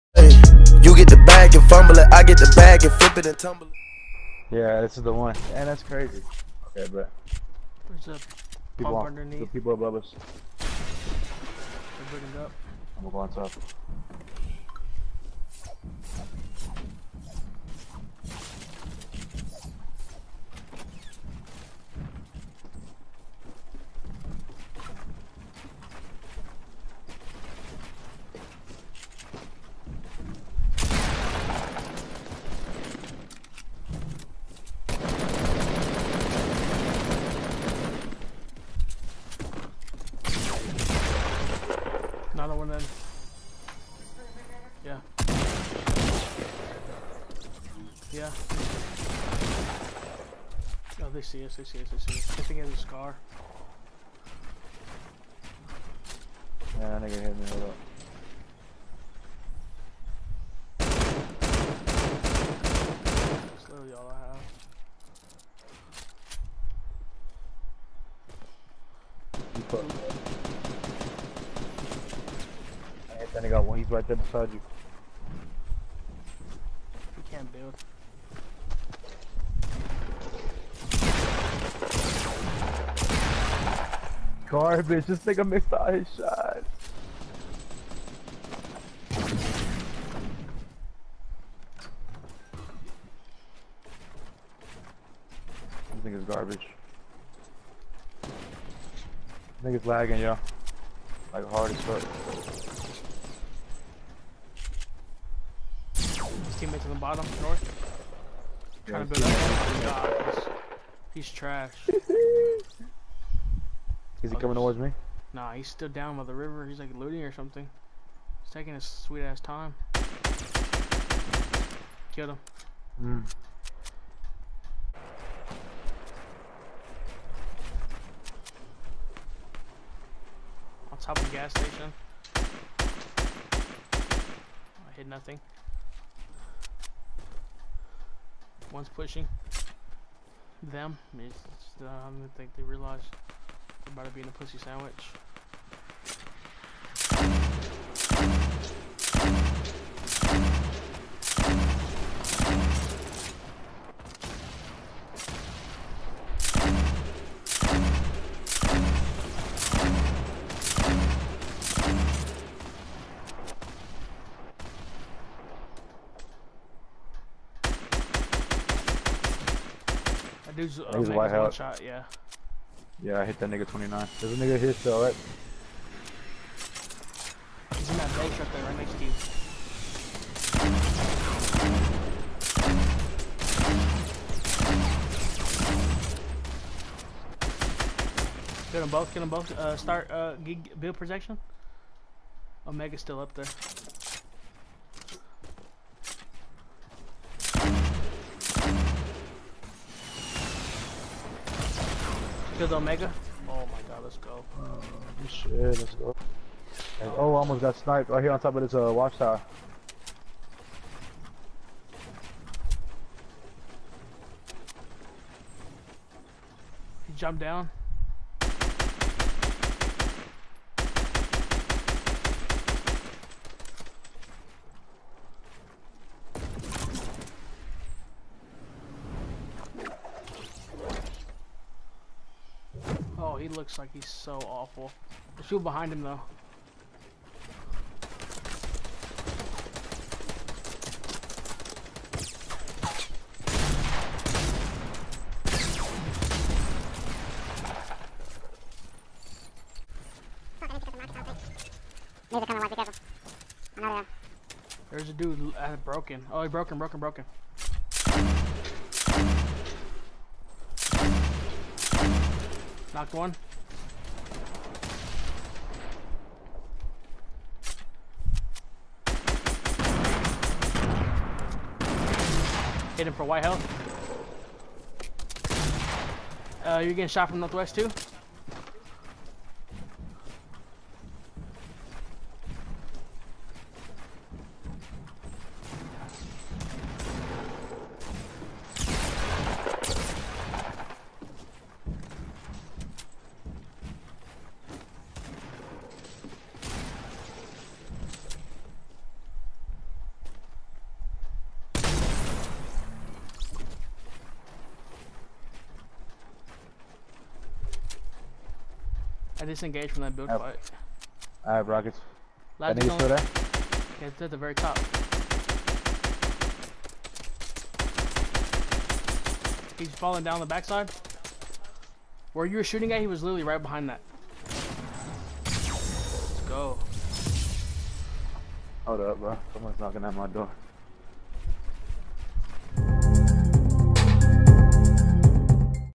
PLAYED DUOS WITH TSM MYTH LOOK A LIKE (Skull Trooper) AND WE WON !!!!
Intro
ME 😏